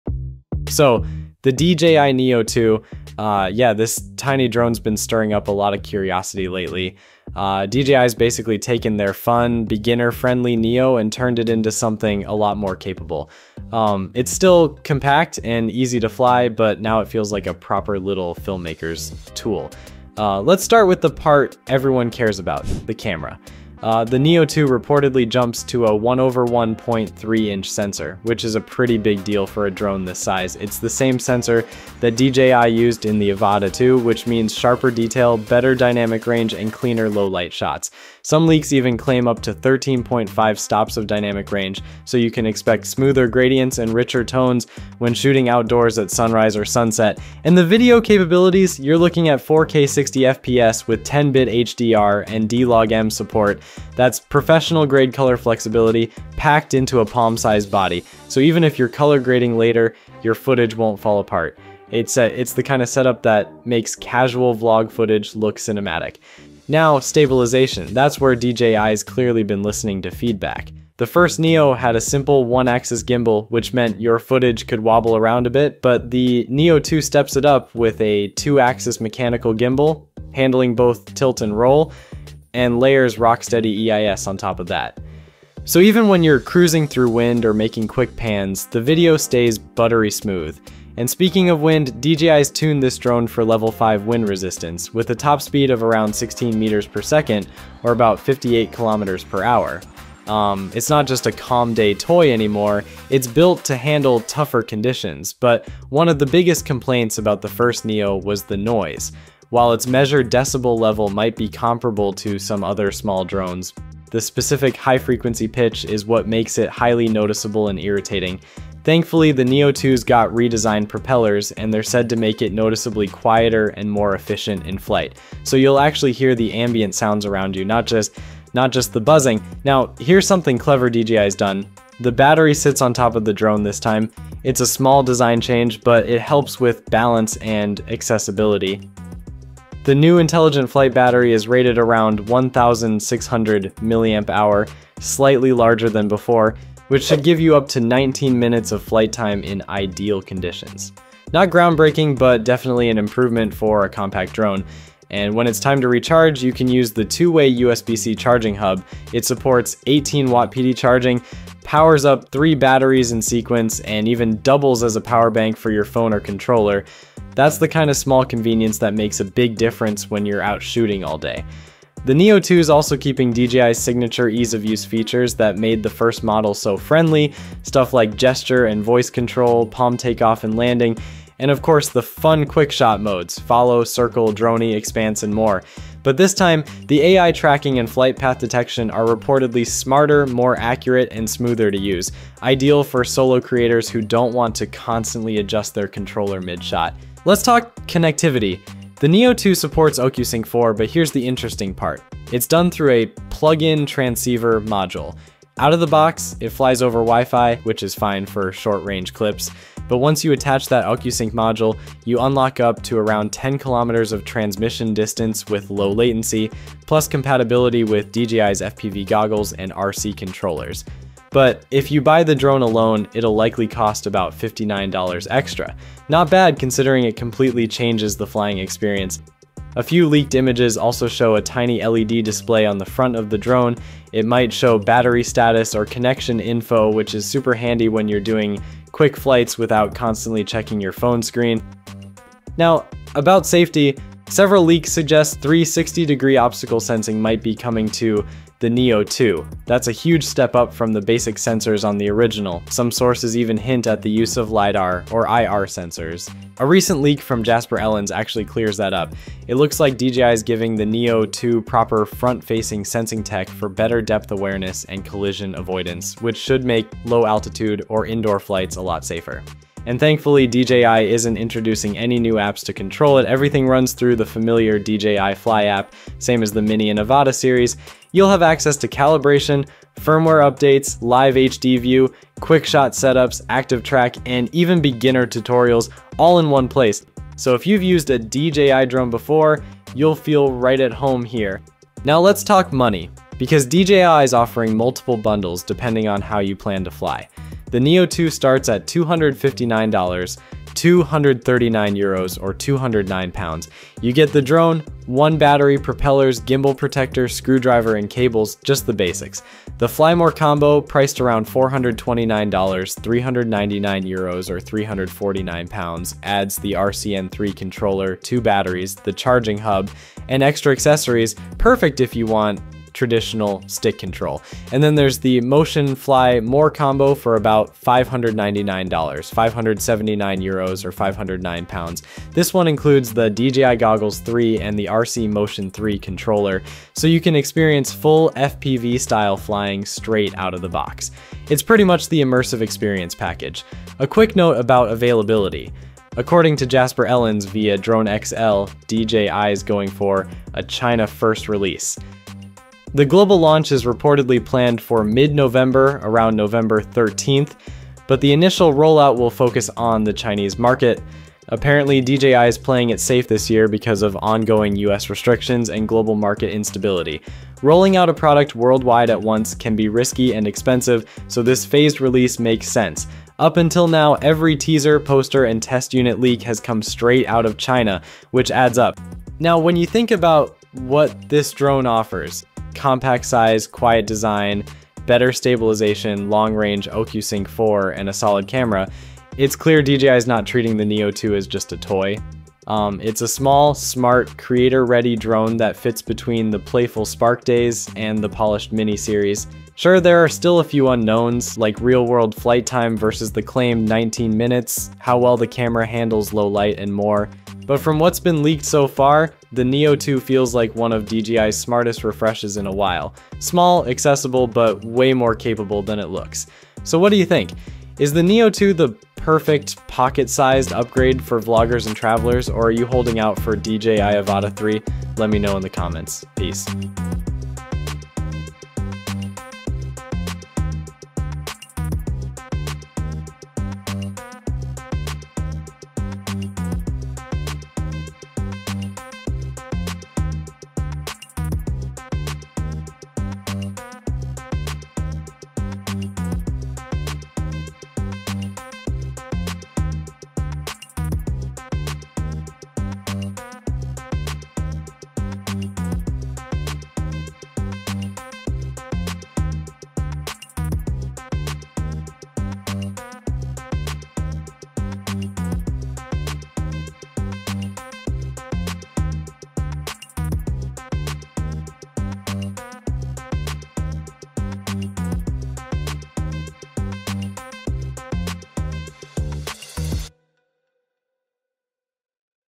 DJI Neo 2 – Complete Scoop on DJI’s Next Compact Drone.
A new DJI Neo 2 leak from Jasper Ellens just confirmed something big — the compact drone is getting front-mounted LiDAR for obstacle detection. But that’s not all; there are still questions about its real flight time, camera upgrades, and transmission system. Let’s break down what’s confirmed, what’s rumored, and what we’re still waiting to find out.

Questions Covered in the video:
    What does the new leak from Jasper Ellens reveal about the DJI Neo 2’s LiDAR system?
    How different is the Neo 2’s design compared to the original DJI Neo?
    Will DJI upgrade the camera or stick with the same 2-axis gimbal setup?
    Could the Neo 2 feature O4 transmission for better range and signal strength?
    What other features or modes might DJI be holding back for launch?